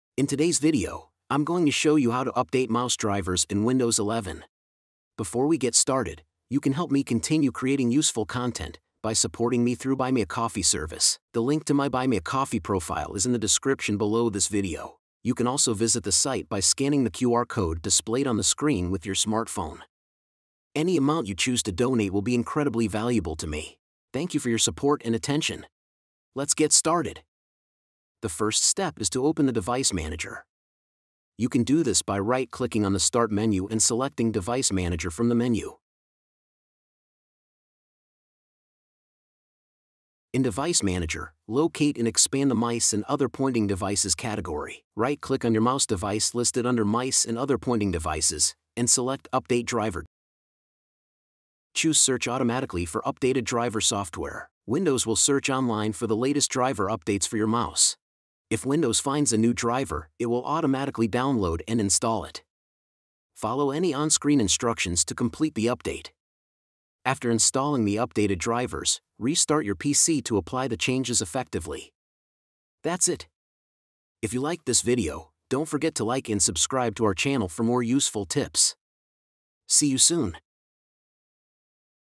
🔄 How To Update Mouse Drivers In Windows 11
🟡BTC: bc1q6lxzusxt27nl25fwcluw7g72uveat2gr7q4as7
🟡ETH: 0xA93CabcE97Ec2c20fc08382283a2CC95d6579EFB
🟡USDT (TRC20): TANkFPkipQ66cFacAe85PiZSq6cEJfBBRQ
🟡USDT (ERC20): 0xA93CabcE97Ec2c20fc08382283a2CC95d6579EFB

In this video, I'll show you how to update mouse drivers on Windows 11. Keeping your mouse drivers up to date ensures smooth and precise cursor movement, improved performance, and compatibility with the latest software updates. Follow this step-by-step guide to update your mouse drivers quickly and easily.

🔧 What You'll Learn:

Updating mouse drivers via Device Manager
Using Windows Update to check for driver updates
Downloading and installing drivers from the manufacturer's website
Troubleshooting common mouse driver issues

💡 Why Update Mouse Drivers?

Ensure smooth and precise cursor movement
Improve overall system performance
Fix bugs and enhance features
Maintain compatibility with new software